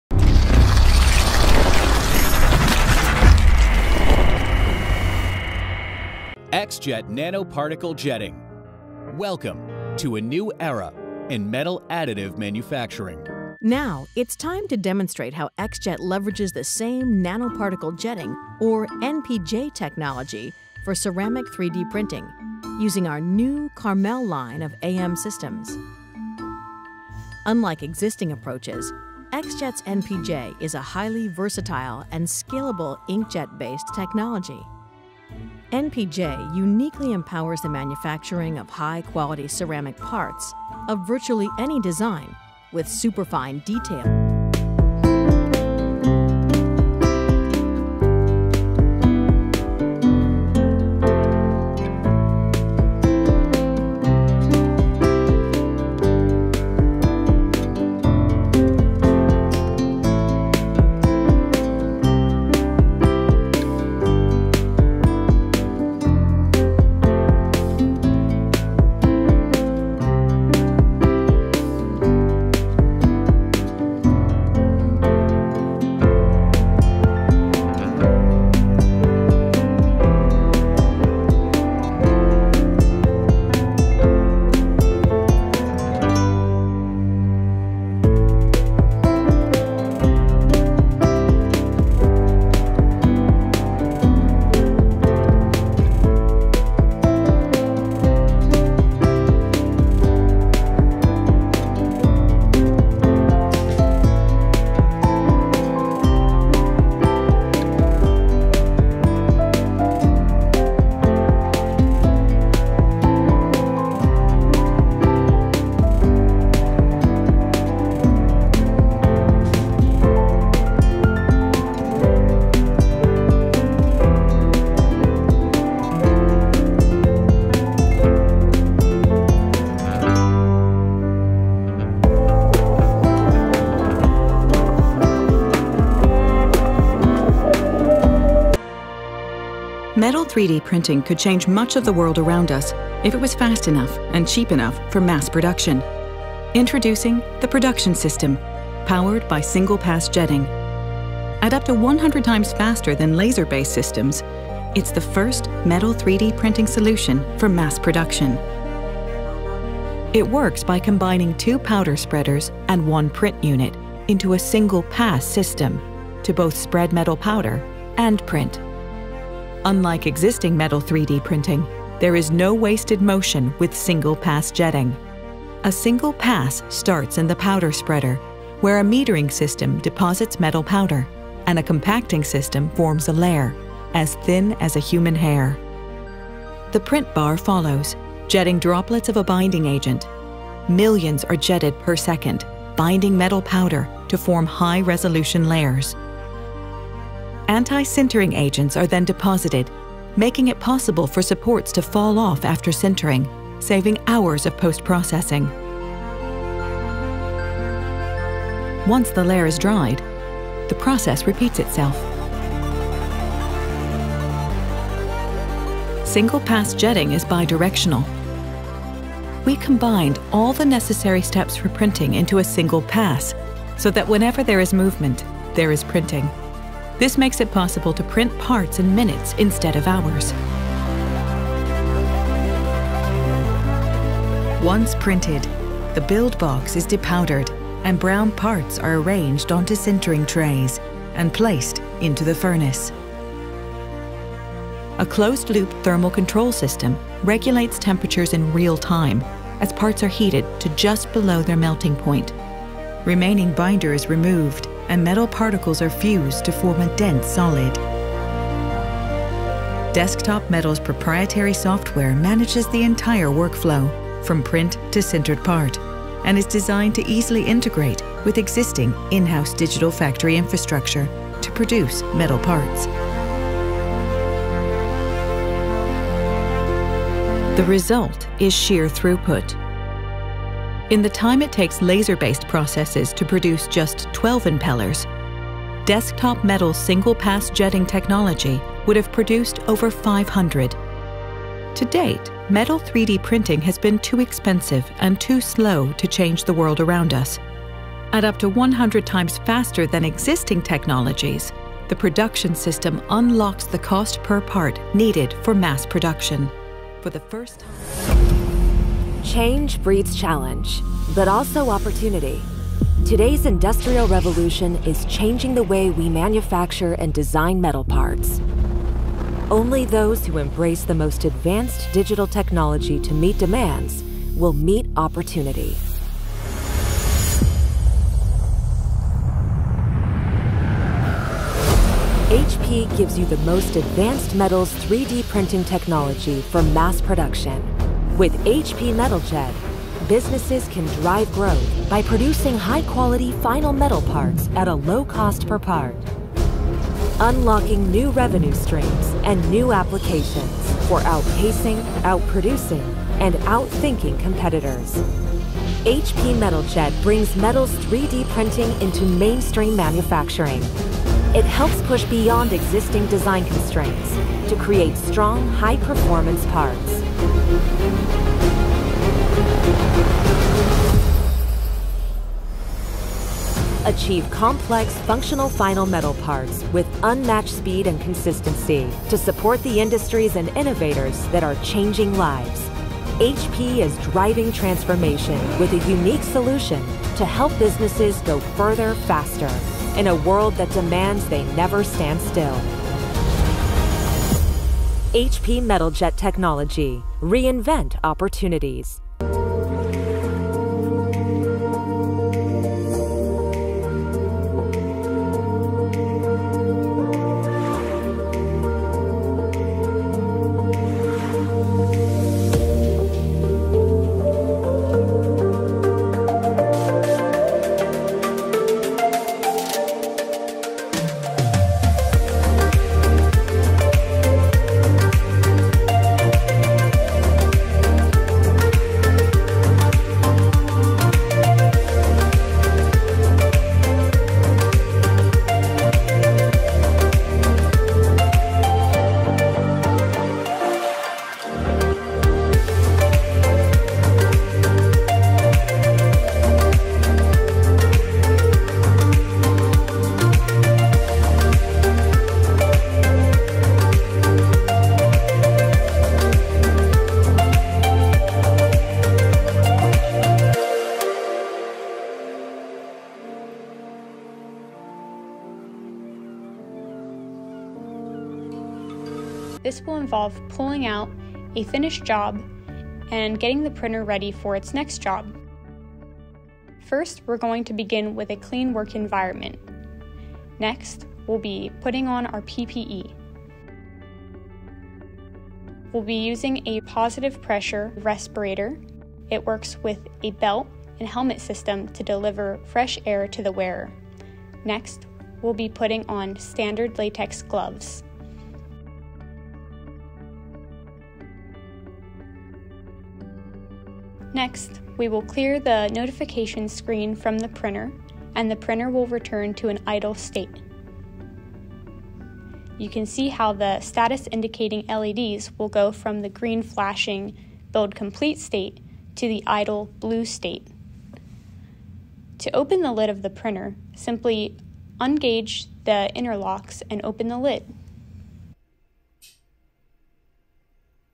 TOP 5 BEST METAL 3D PRINTERS 2023
5️⃣. XJet Carmel 1400C
►Amazon US

4️⃣. Desktop Metal Production
►Amazon US

3️⃣. HP Metal Jet
►Amazon US

2️⃣. Sharebot metalONE
►Amazon US

1️⃣. Xact Metal XM200C
►Amazon US